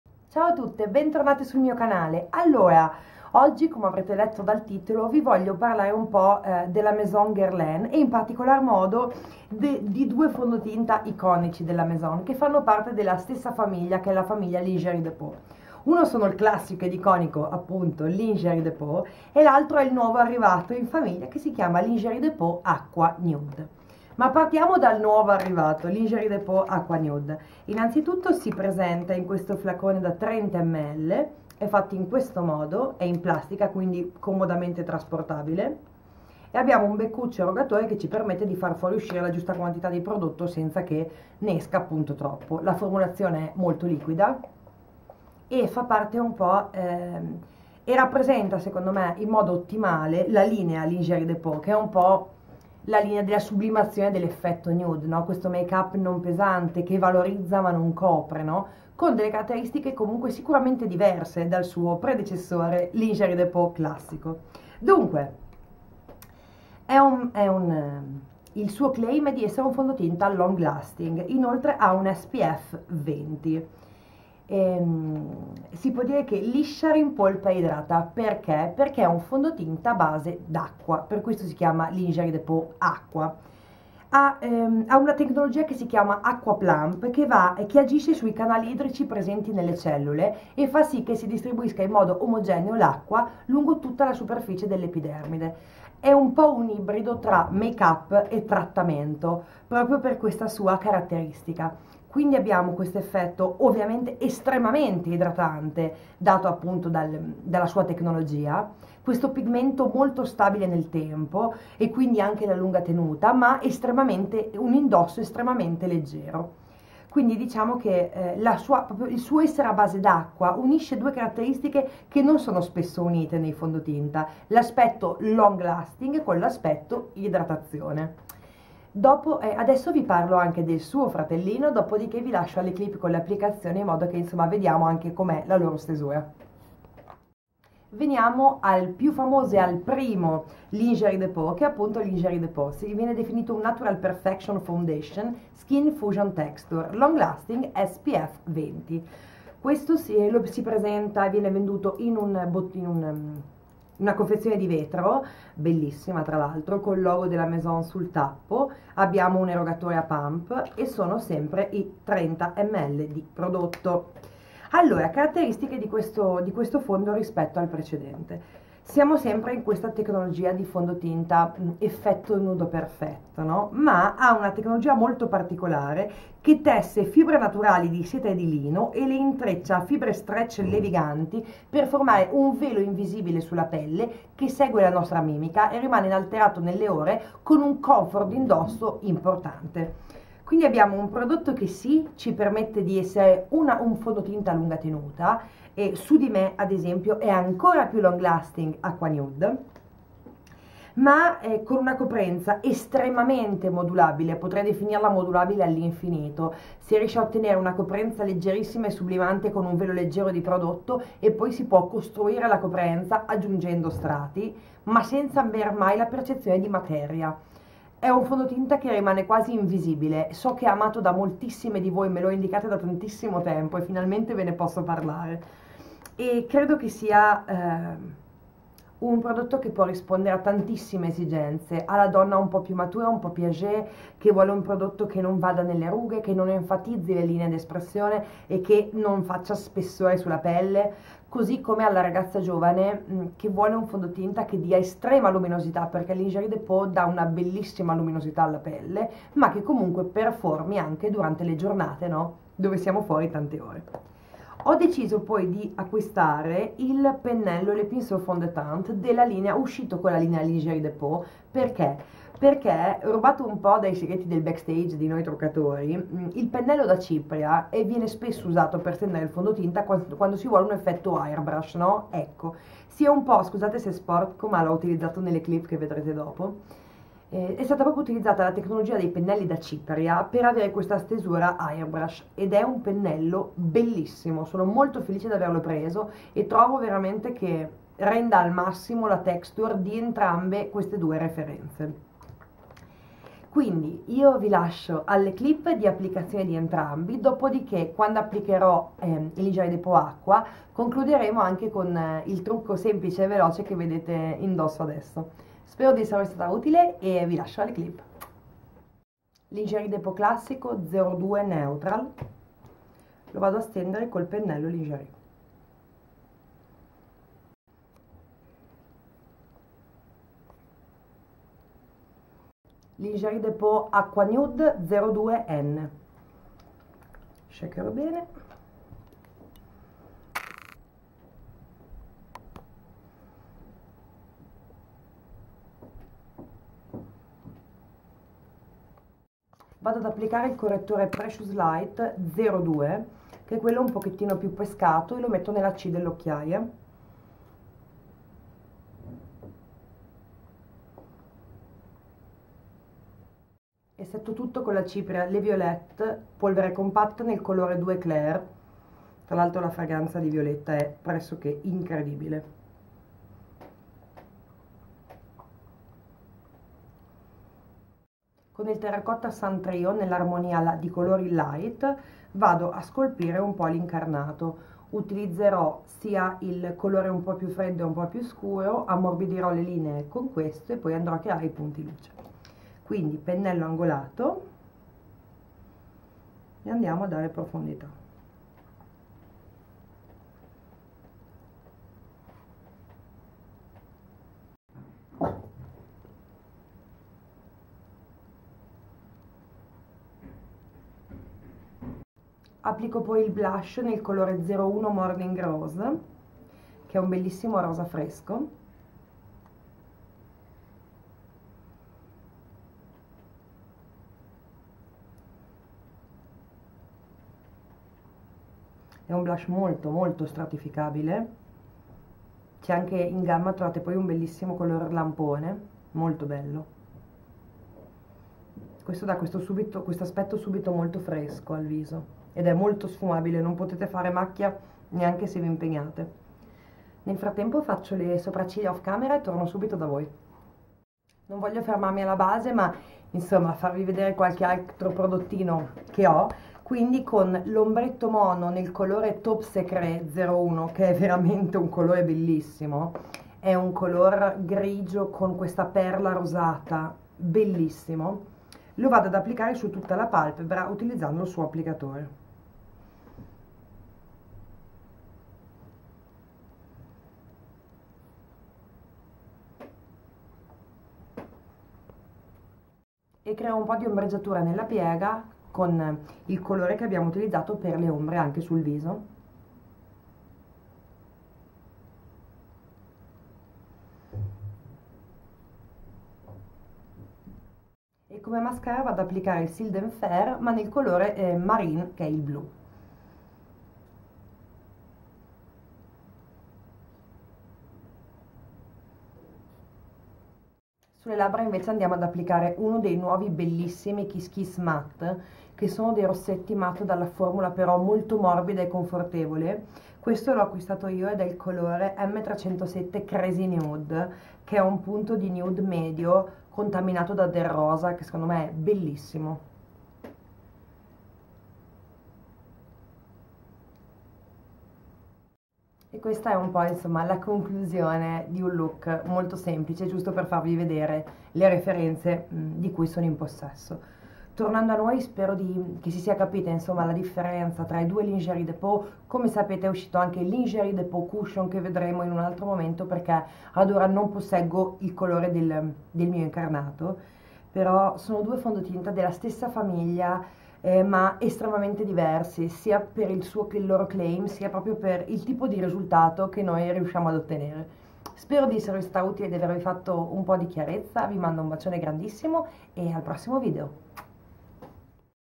GUERLAIN LINGERIE DE PEAU & LINGERIE DE PEAU AQUA NUDE:Vediamoli insieme | Giulia Cova
Nuovo video ragazze e ragazzi ed oggi vediamo insieme l'iconico fondotinta della Maison Guerlain Lingerie de peau ed il nuovo arrivato,Lingerie De peau Aqua Nude.
Buona visione!

PRODOTTI UTILIZZATI:
-Ligerie Aqua Nude